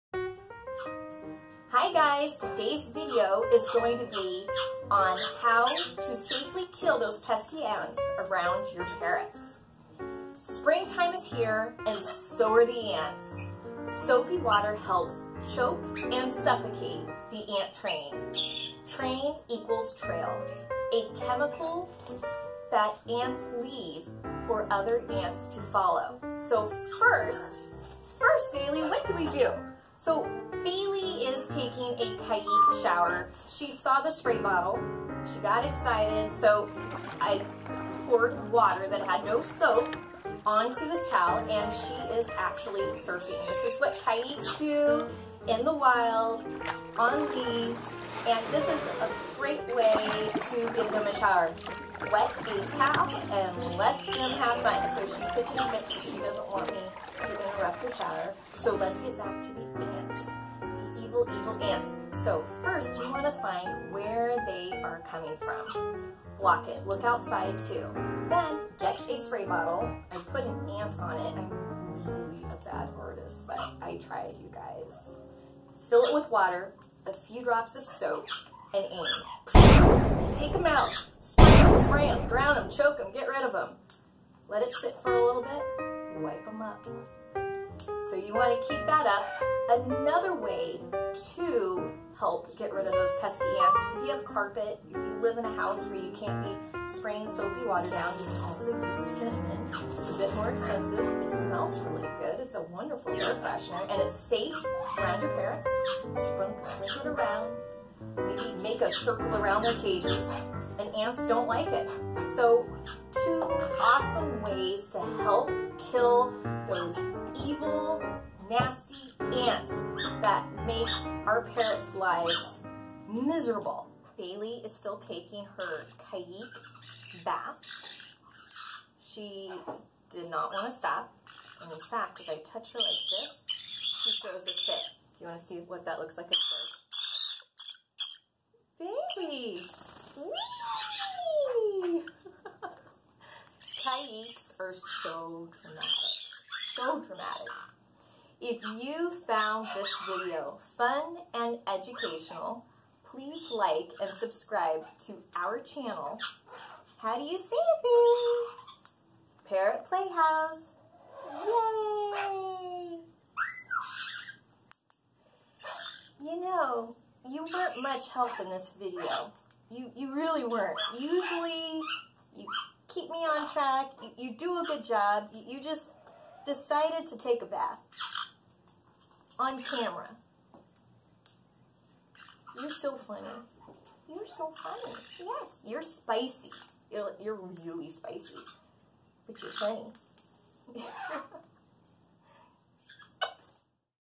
How to Kill Ants Pesticide Free | Bird Care
Little side note about 🐜:🐜 don't have lungs. They have little tubules that air gets into their body - by DIFFUSION. So when those tubes are covered with something that doesn’t allow air to pass they sufficate. Soapy water gives JUST enough surface tension to ‘stick’ and not let air through. It helps cut through the fat and grease of their "smell trails" they use to follow each other. So it begins to clean up and break that down. ALL SAFELY 🌻 NO CHEMICALS🐦❤️